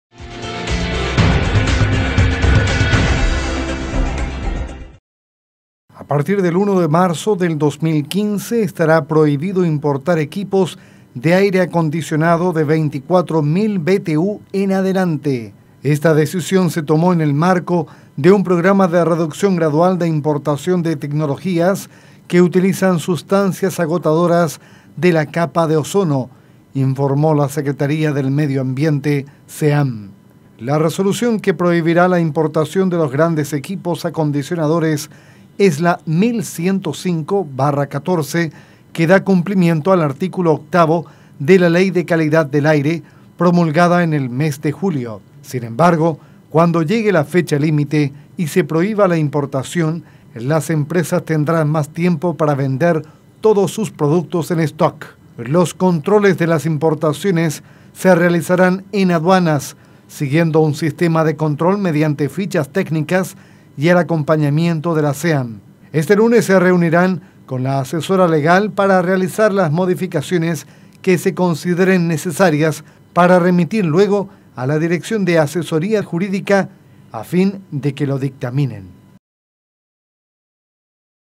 Limitarán compra de equipos de aire acondicionado
La Secretaría del Ambiente (Seam) informa que inició un plan para ayudar a la capa de ozono, reduciendo la importación de equipos de aire acondicionado desde 2015.